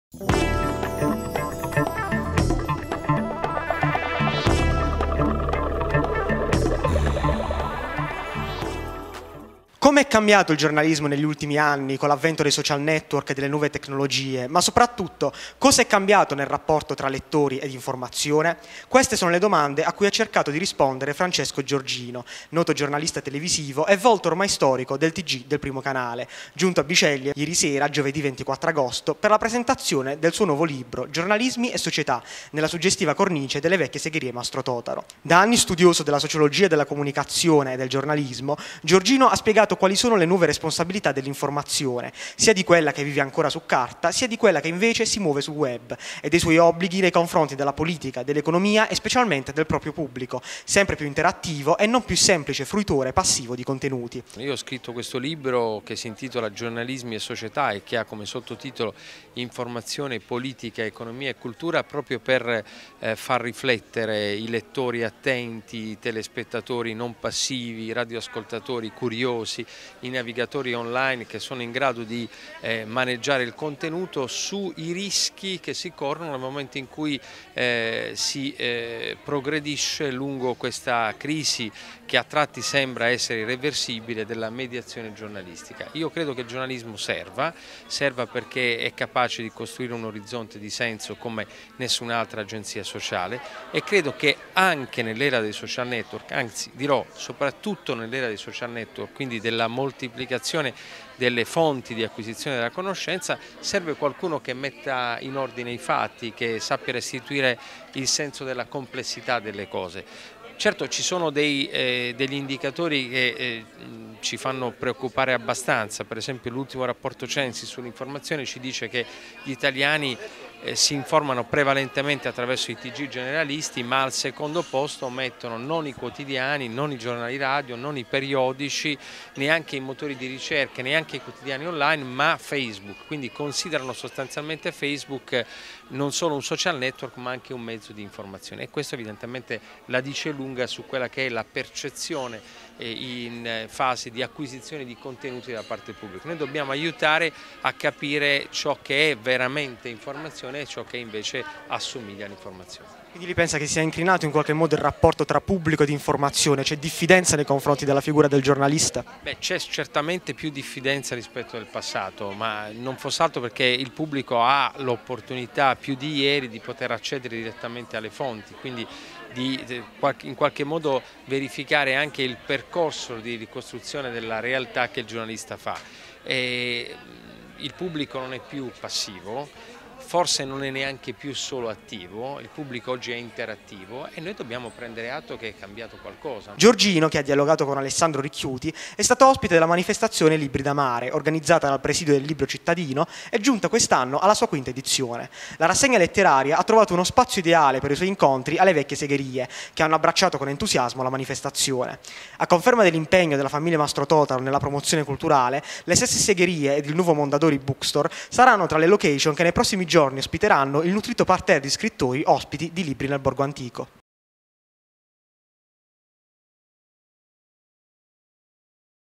“Giornalismi e società” a Libridamare: Francesco Giorgino spiega come è cambiata l’informazione
Come è cambiato il giornalismo negli ultimi anni, con l'avvento dei social network e delle nuove tecnologie, ma soprattutto cosa è cambiato nel rapporto tra lettori ed informazione ? Queste sono le domande a cui ha cercato di rispondere Francesco Giorgino, noto giornalista televisivo e volto ormai storico del Tg del primo canale, giunto a Bisceglie per sera, giovedì 24 agosto, per la presentazione del suo nuovo libro "Giornalismi e società" nella suggestiva cornice delle Vecchie Segherie Mastrototaro.